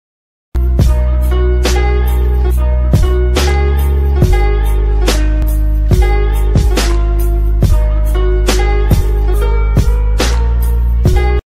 Calm
A walk cycle I did to take my mind off stuff

The character in this one isn't colored, but his name is Oil Slick. He's a coyote who lives in the Fall Ground area, which is a radioactive wasteland (hence his multiple eyes and his ability to have extremely flammable fur that can burn without him getting hurt).